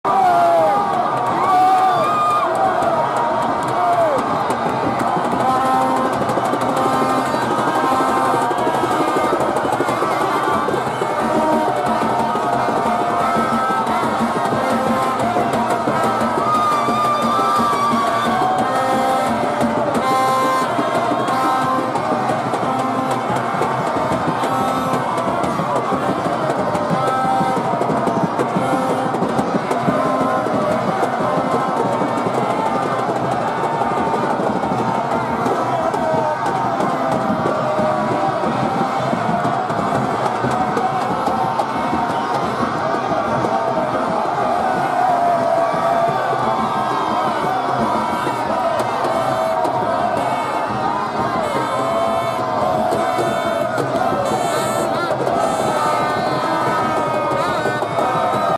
REAÇÃO DOS ADEPTOS APÓS O GOLO FRENTE AO EGITO
Euforia nas bancadas onde se encontravam os apoiantes da seleção nacional, cada qual dançava e ao seu ritmo até o árbitro reverter a decisão.

O verdadeiro "Man Of The Match" acabou com a festa minutos depois!